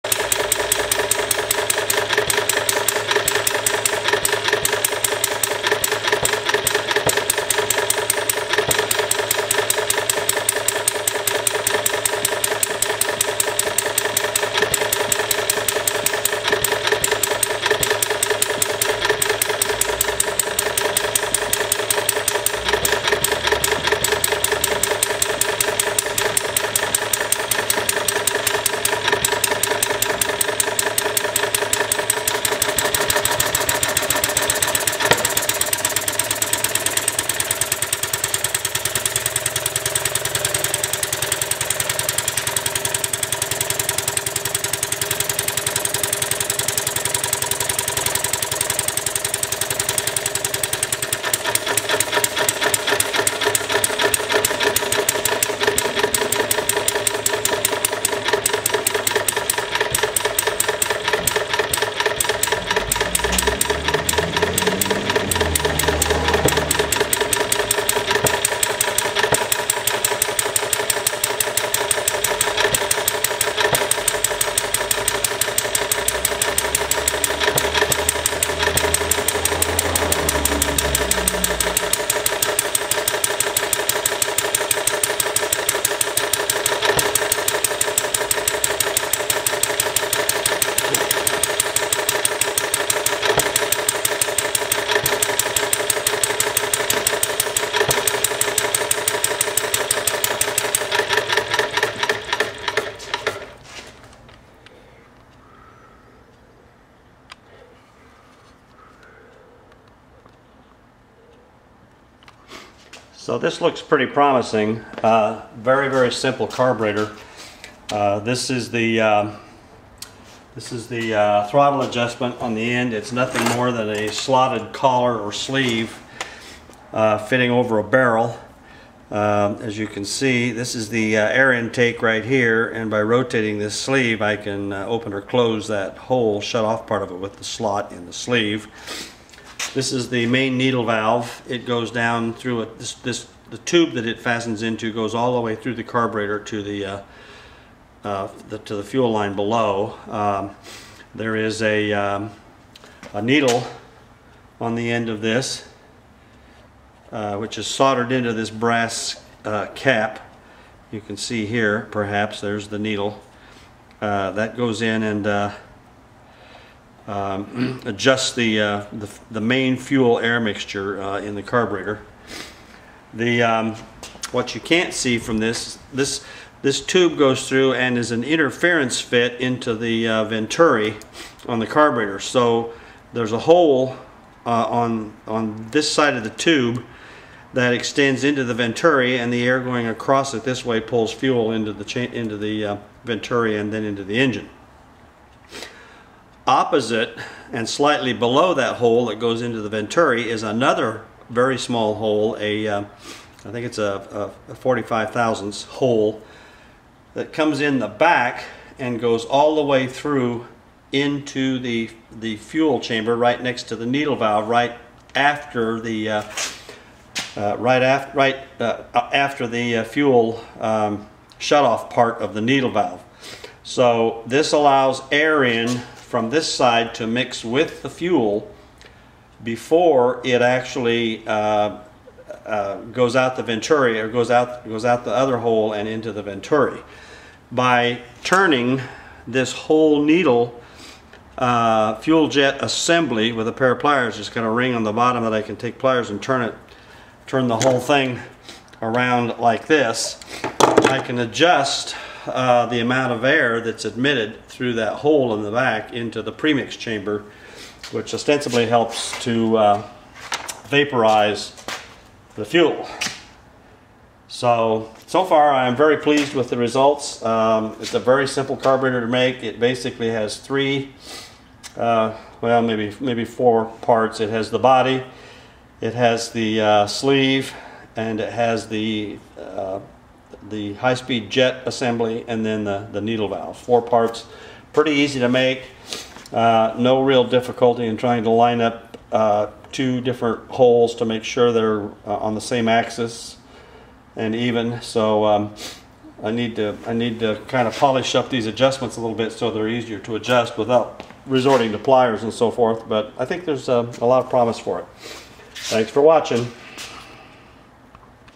New carburetor for my plumbing parts engine
This is my new carburetor design.  It's the simplest and easiest to build I've seen so far.  The mixer assembly includes the mixer tube and the needle valve.  It is an interference fit with the 1/8" air passage in the body and forms a venturi by partially obstructing it.  The fuel orifice faces in toward the center of the venturi.  The throttle is a split sleeve on the back of the carburetor.  Speed is controlled by rotating the sleeve which moves the slit over a hole drilled into the body from the side.  There is also a premix or air bleed hole opposite the fuel orifice which lines up with a small hole in the body of the carburetor.  By rotating the entire mixer tube slightly, the air bleed opening can be adjusted.